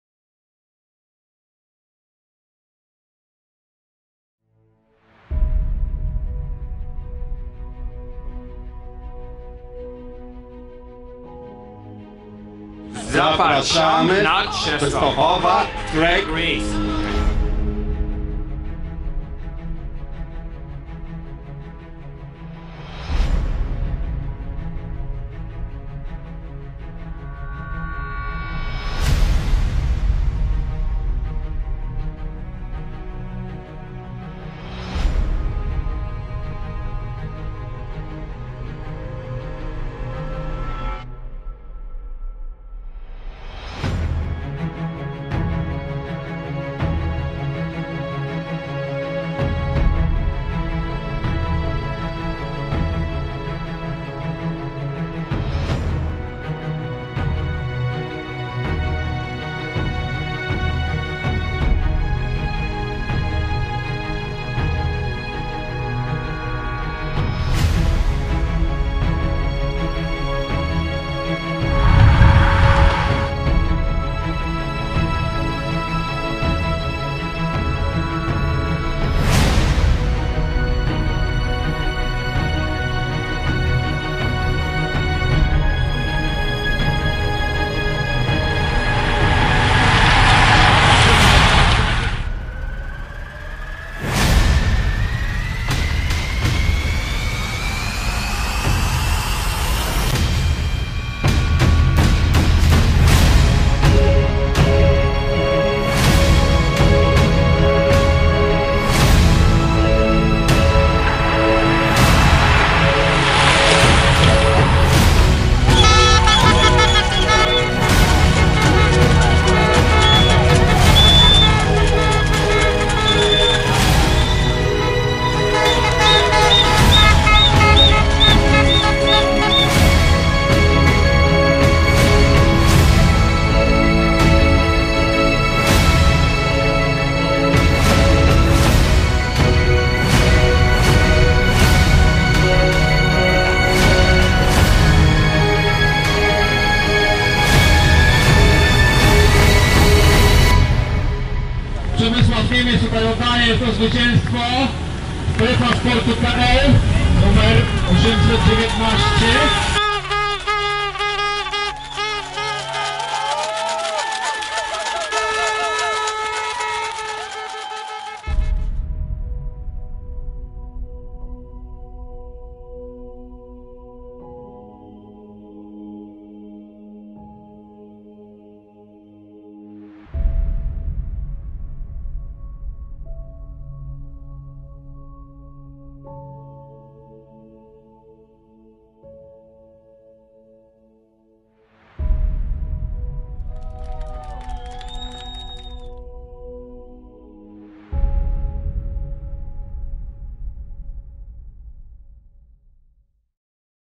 CZĘSTOCHOWA TREK RACE 2019
Druga edycja Częstochowa TREK Race, klasyka szosowego dla amatorów, który jest zarazem oficjalnymi Mistrzostwami Częstochowy.
Wśród startujących gwiazdy z Polski i nie tylko jak: Jens Voigt, Przemysław Niemiec, Cezary Zamana i wielu innych.
Ponad 500 osób na starcie!